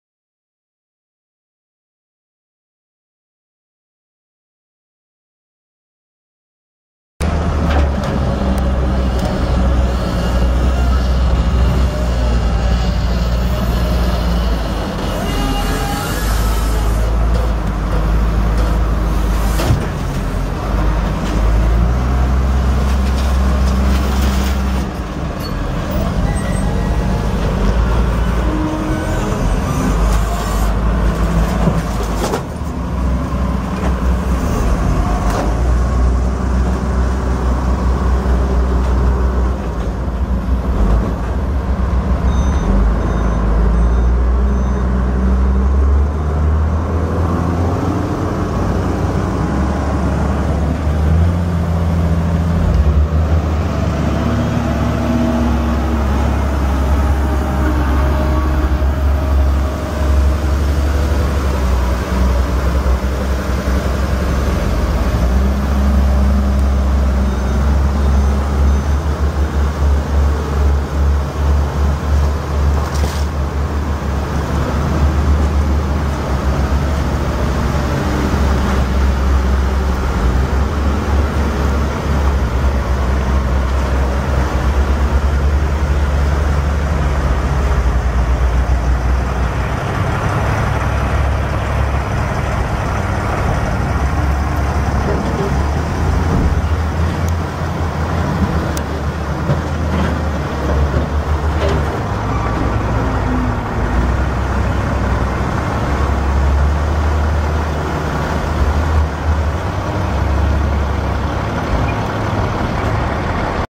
1987 Komatsu WA150 wheel loader for sale | sold at auction November 20, 2014
1987 Komatsu WA150 wheel loader
5,412 hours on meter
Komatsu SD695L-1 six cylinder diesel engine
Serial 17898
Powershift transmission
3F-3R gears
Enclosed cab
Spring suspension seat
92"W bucket
Weld-on cutting edge
Chain hooks
Rear counter weight
17.5-25 tires
17,330 lbs. base operating weight
Battery cover missing
Seat torn
Side window cracked
Serial 10282